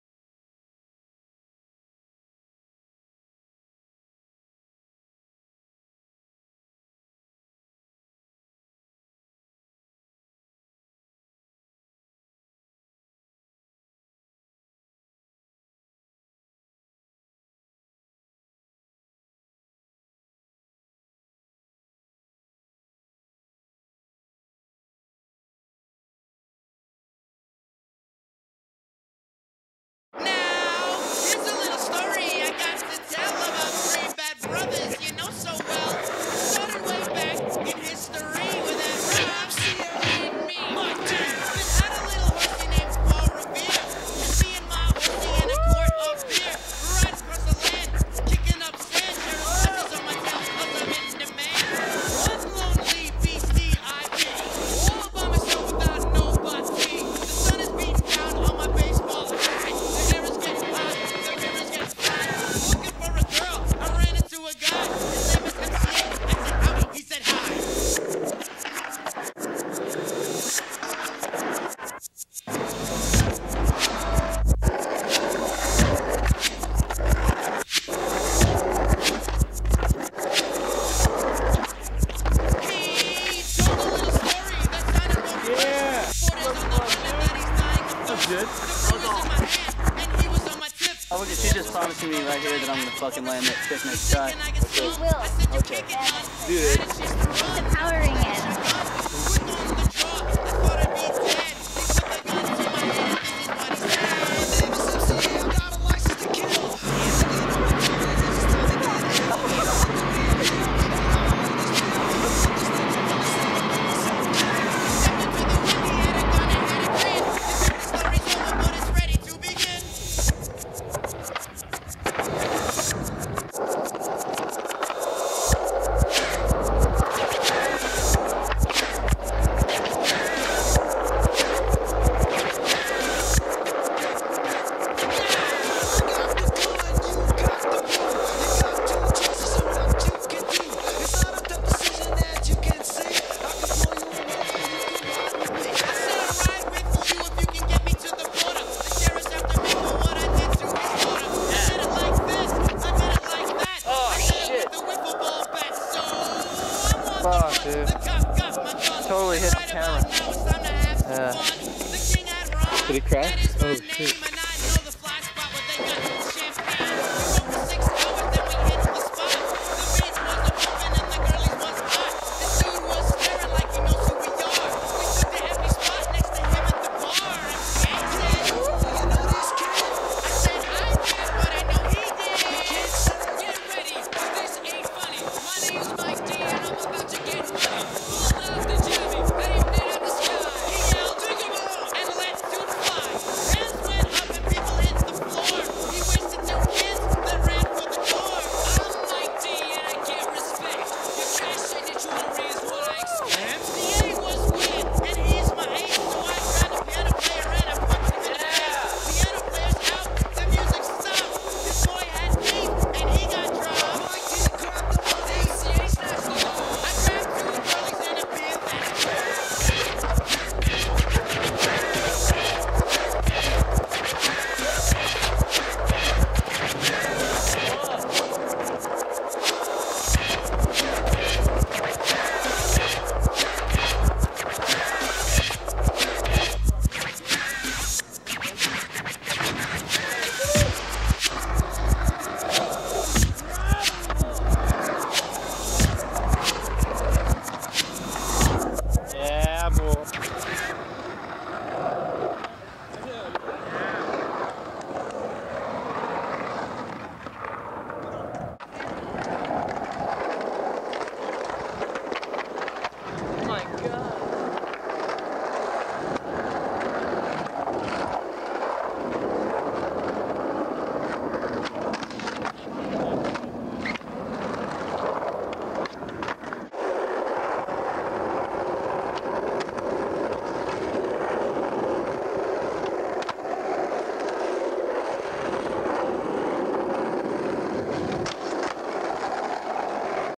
San Jose '95- '96
The first of a few edits to come. This one featuring Jeff Kim, Aaron Van Den Bulke, Noah Baxter, Doug Shoemaker, Tim Branch, Mark Johnson, Chris Avery, Ave Bonechitta, Pancho Moler, Steve Caballero, and others (yo mama included).